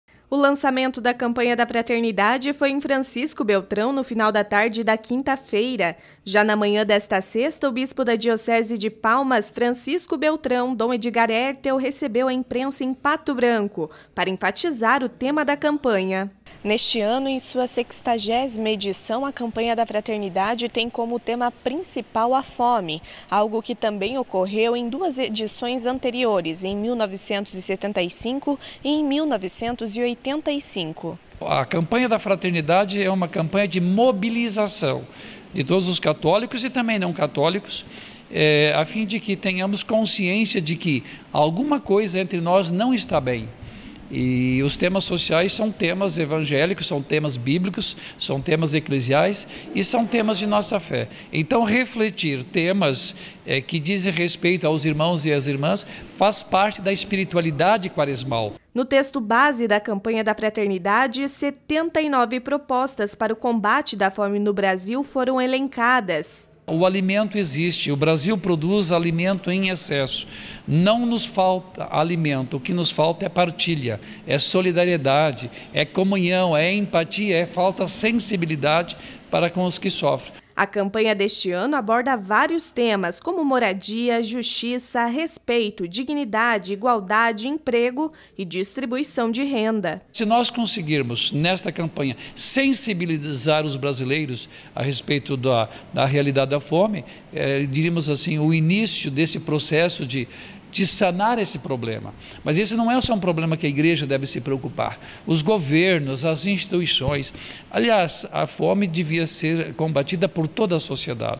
Campanha da Fraternidade 2023: "Dai lhes vós mesmo de comer."
Foi lançada na diocese de Palmas - Francisco Beltrão a campanha da Fraternidade deste ano. O tema é Fraternidade e Fome e o lema: Dai lhes vós mesmo de comer